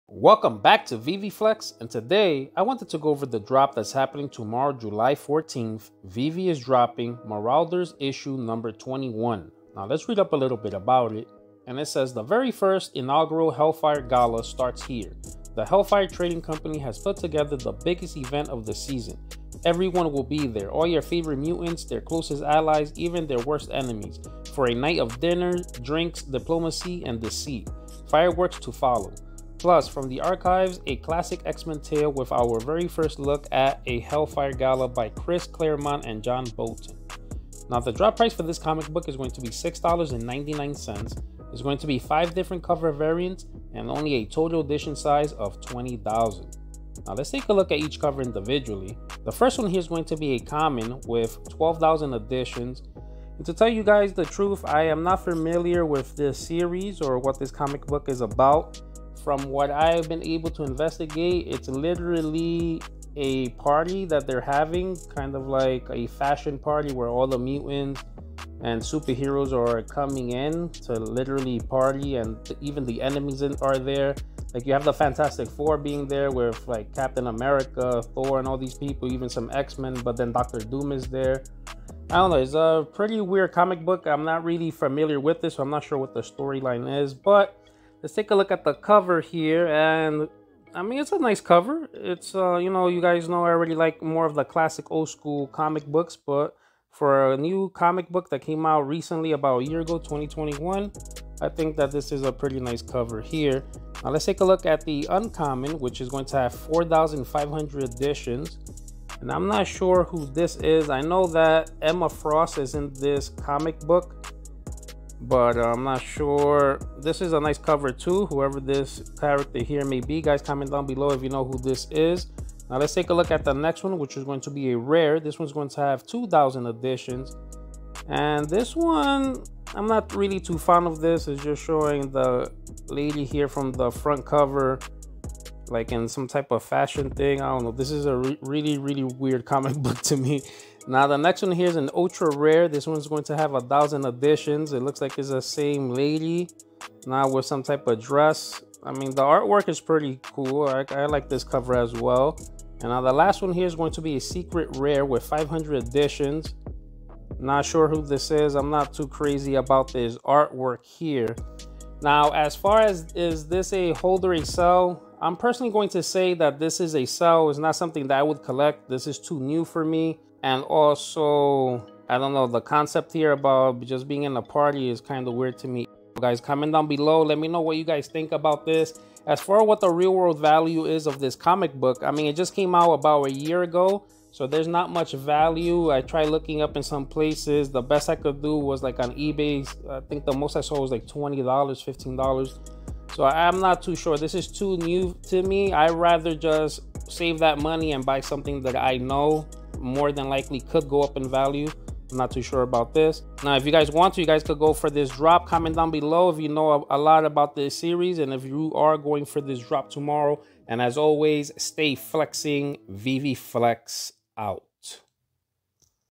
VeVe Drops Marauders #21 Digital Comic NFT, is it a HOLD or SELL?
The very first inaugural HELLFIRE GALA starts here! The Hellfire Trading Company has put together the biggest event of the season! Everyone will be there — all your favorite mutants, their closest allies…even their worst enemies — for a night of dinner, drinks, diplomacy and deceit. Fireworks to follow. Plus: From the archives, a classic X-Men tale with our very first look at a Hellfire Gala by Chris Claremont and John Bolton!